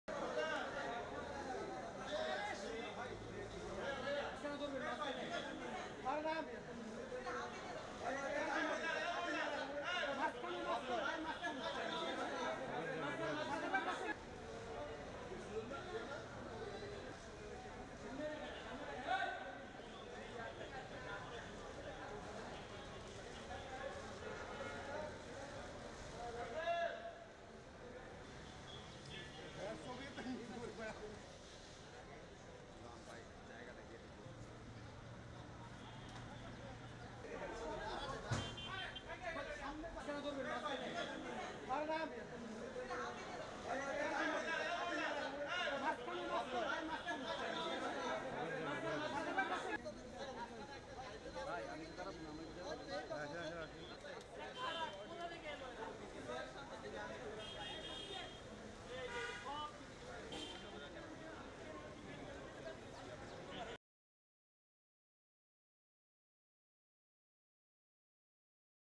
ব্যর্থ মিশন শেষে দেশে ফিরলো শান্ত-তামিমরা | Bangladesh Cricket Team | Nagorik TV
NAGORIK TV
Address: Kha-53, Nikunja-2, Dhaka-1229